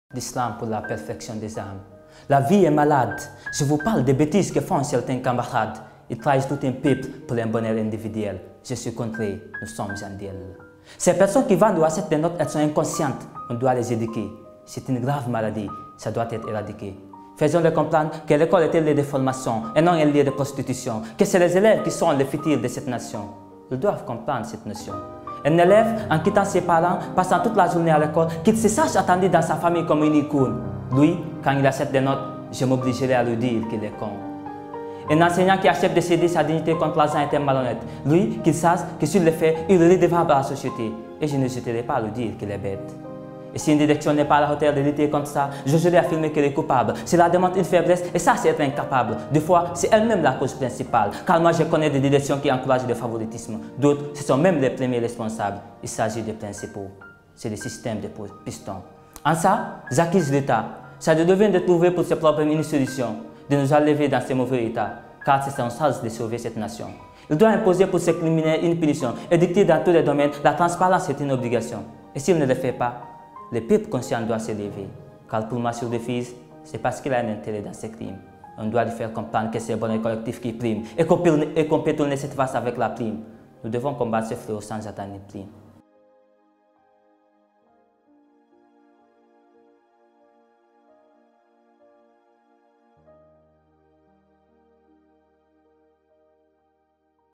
Guinée : le ras-le-bol d'un jeune slameur contre la corruption
Ces dernières années, le phénomène de la corruption a pris une ascension inquiétante en République de Guinée. Selon l’indice de perception de la corruption de 2020 (publié par Transparency International) plusieurs pays d’Afrique subsaharienne figurent dans le top 50 des pays les plus corrompus du monde.

Un classement qui interpelle jeunes quinze (15) jeunes artistes (poètes et slameurs) de la ville de Labé, à 440 kilomètres de la capitale Conakry. Après avoir bénéfice d’une session de formation en art artistique à travers un appui d’Open Society For West Africa (OSIWA), ces jeunes ont décidé mettre leur plume au service de la lutte contre la corruption.

Dans ces deux premières vidéos d’une série de 15 quinze vidéos, le premier Alpha Oumar Diallo élève en classe de terminale dénonce la vente de notes par certains professeurs et quant à Ramatoulaye Diallo également élève, elle interpelle les citoyens sur la gravité de la corruption et son impact dans le développement socio-économique d’une nation.